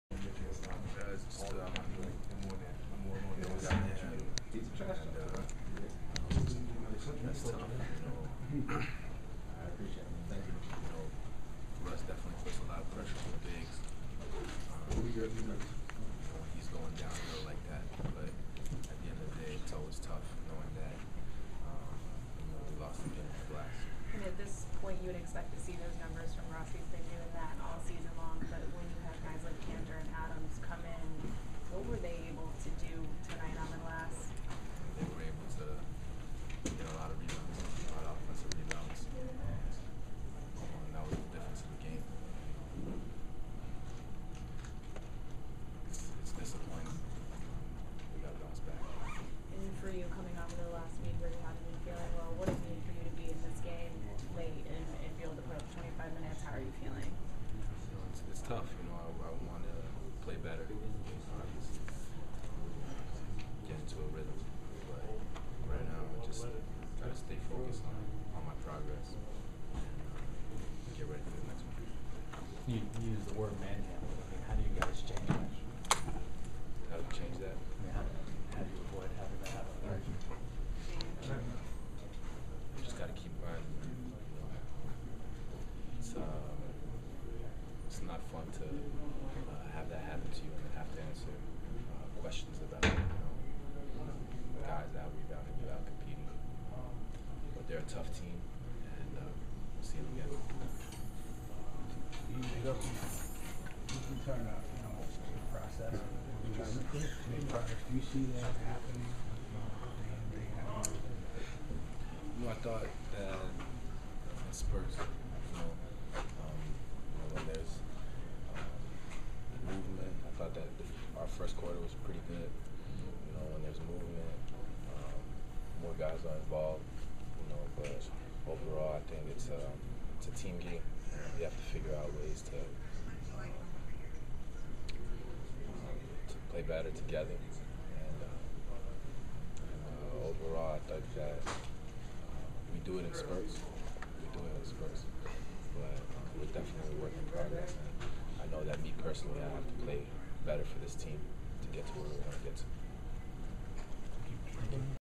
Joakim Noah on his health, pressure Russell Westbrook puts on defense
Joakim Noah of the New York Knicks discusses the pressure Russell Westbrook puts on the defense, the team getting "manhandled" by Steven Adams and Enes Kanter, and more after a loss to the Oklahoma City Thunder at Madison Square Garden in New York City